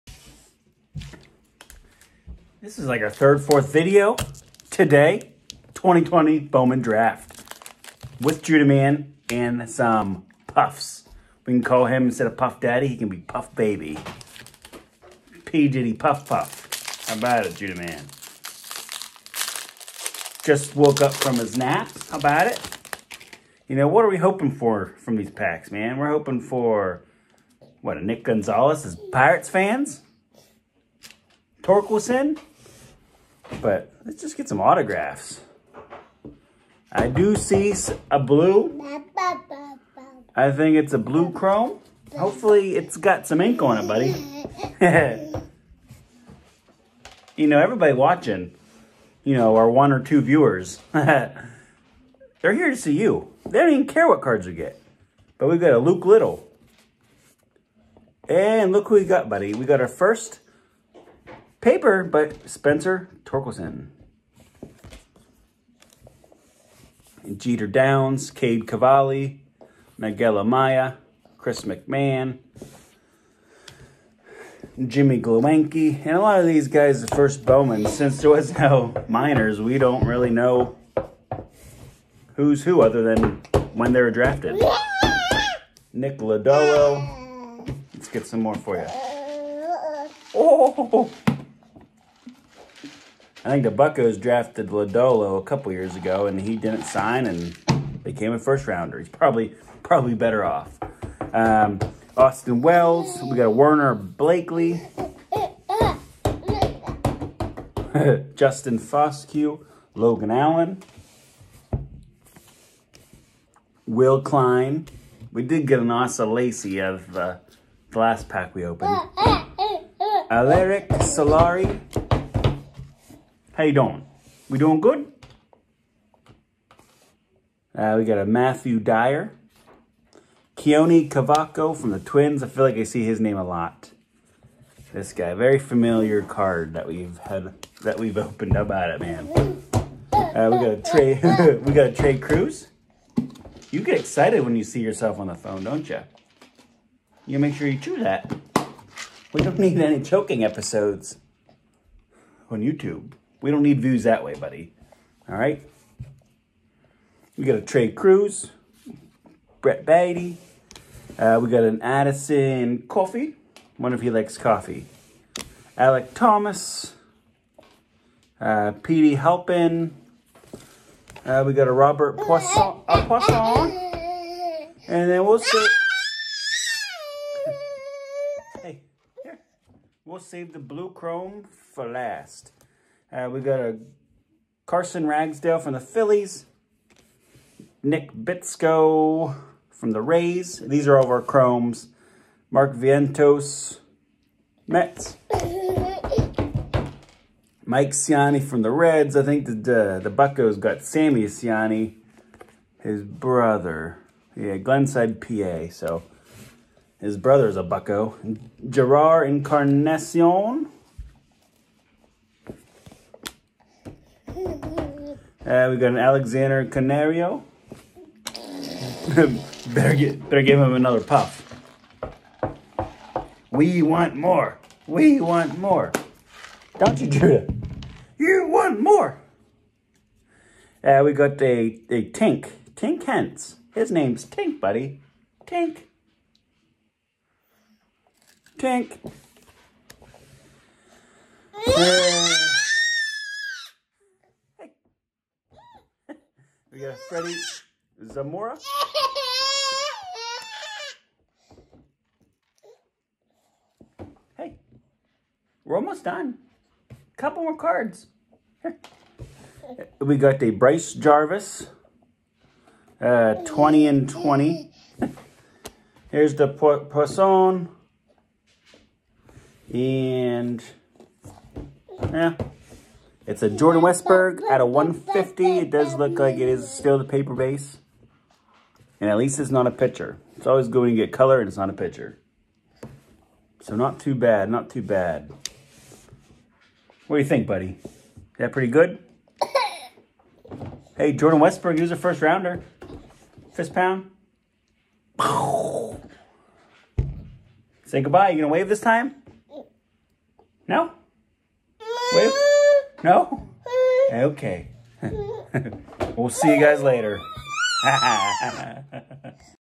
2020 Bowman Draft with Judahman do we finally get a Torkelson
Judahman rips 1 pack of Bowman draft